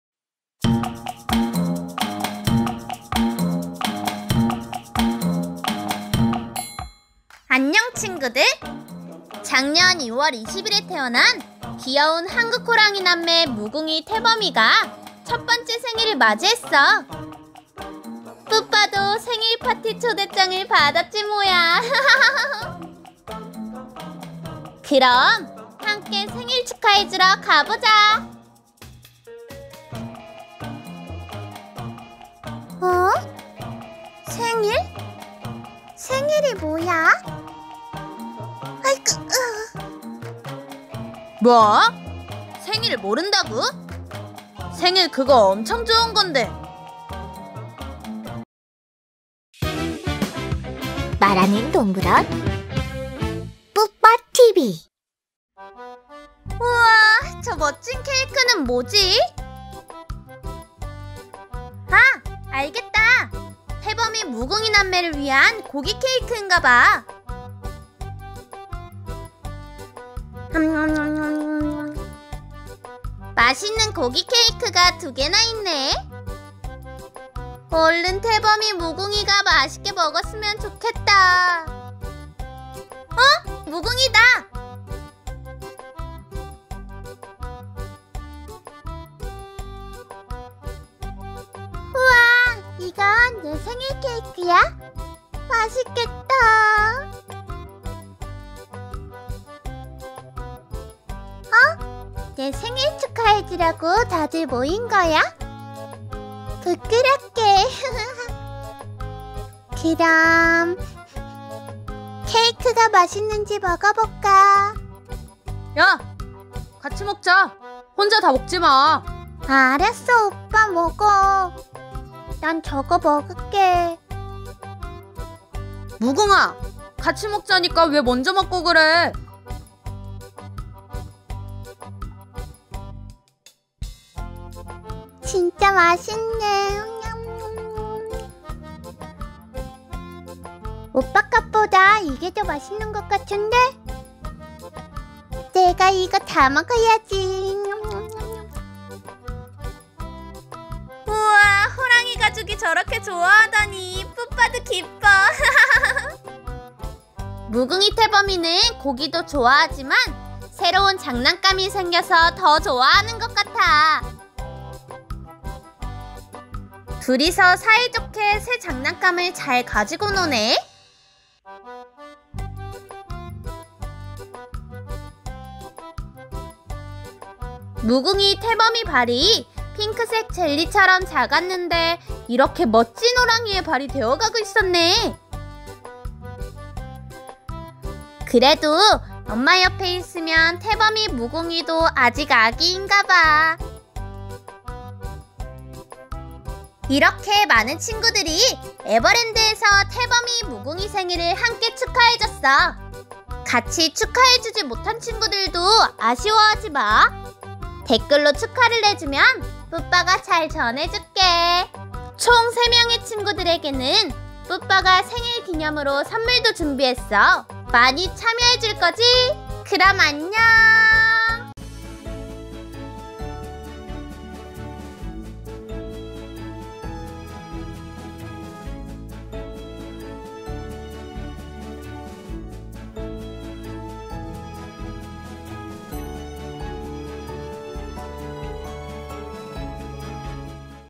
[🐯🎂이벤트] 아기호랑이가 벌써 생일이라구? 태범, 무궁이 생일 파티 현장에 가다 ㅣ 에버랜드 타이거밸리(Everland Baby tiger)
칭구들 안녕~ 뿌빠야!
저번에 태범이 무궁이 생일 파티에 초대받았다고 자랑했지?

귀염귀염 아기호랑이들이 이제 무럭무럭 자라서
타이거밸리의 늠름한 어린이가 되어 있지 뭐야!
뿌빠도 너무 기뻐서 많이 축하해주고 왔는데~
혹시 아직 축하 못한 친구들!

무궁이, 태범이 생일을 축하하는 댓글을 써 준 3명의 친구들에게
귀여운 호랑이 굿즈를 선물로 줄게! (~3/4)
그럼 많은 축하 부탁해!뿌빠뿌빠 💨💨